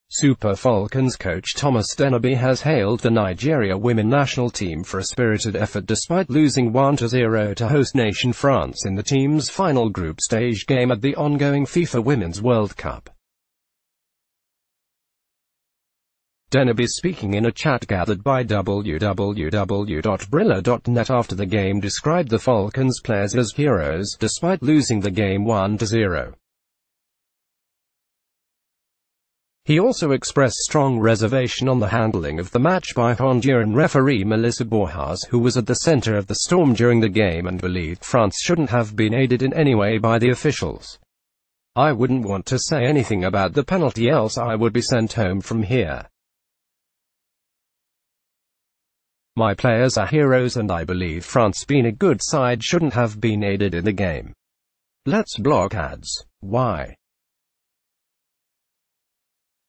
Sport News| Dennerby Praises Falcons in spite of defeat to France, expresses reservation on offic...
Sport News| Dennerby Praises Falcons in spite of defeat to France, expresses reservation on officiating - Latest football news in Nigeria